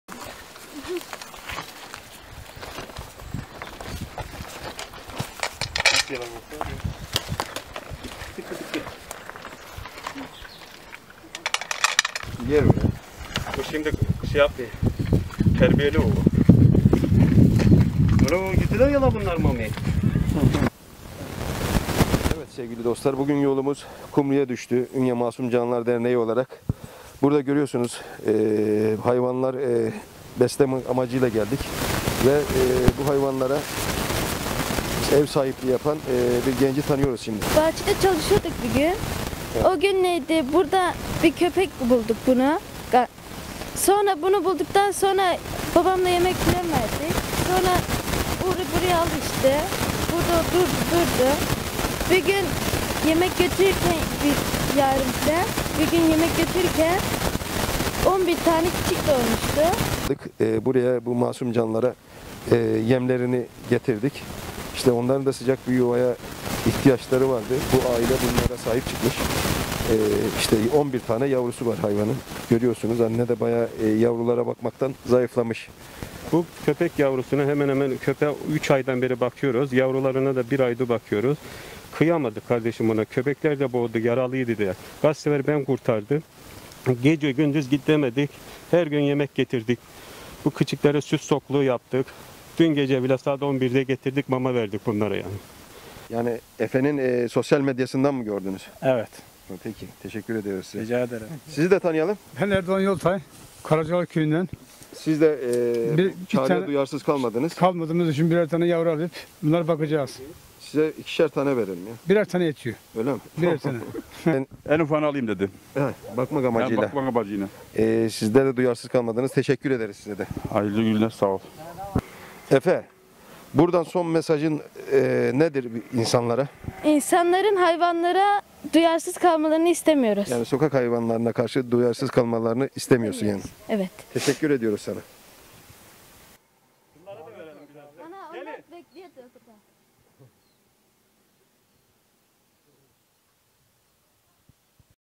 Yardım Edin Çağışına Sessiz Kalmadılar
Kumrulu ortaöğretim öğrencisi Efe Kerim Erecek, babası ile birlikte bahçeye çalışmaya gittikleri sırada tanıştığı köpeğin bir zaman sonra yavrularının doğmasının karşısında şaşkınlığını gizleyemedi. Yavrulara mama yetiştiremeyen Efe Kerim’İn yardımına Ünye Masumcanlar Sokağa Terk Edilmiş Hayvanları Koruma Derneği yetişti.

Kumrulu ortaöğretim öğrencisi Efe Kerim Erecek babası ile birlikte bahçeye çalışmaya giderken yaralı bir köpeğin peşlerine takılmasıyla her şeyin başladığını söyledi. Sevimli dostuna mama getirdi. Gördüğü manzara karşısında şaşkınlığını gizleyemedi.

Efe Kerim Erecek ,“ bahçede çalışıyorduk bir gün .sonra köpek bulduk. Bulduktan sonra babamla yemek falan verdik. Birgün yemek verirken 11 tane yavru köpek doğurmuştu" dedi.

Yavru köpeklerle birlikte artan boğaz derdine yetişemeyen Efe Kerim ve babası çareyi Ünye Masumcanlar Sokağa Terk Edilmiş Hayvanları Koruma Derneğini aramakta buldu.

Efe’nin sosyal medyadan yaptığı sahip çıkın çağrısına cevap veren vatandaşlar soluğu yavruların yanında aldı.

Ünye Masumcanlar Sokağa Terk Edilmiş Hayvanları Koruma Derneği başkanı Hakan Yıldız ve Derneğin Yönetim Kurulu Üyesi Murat Özkaya Efe’nin duyarlı davranışını taktir ettiler.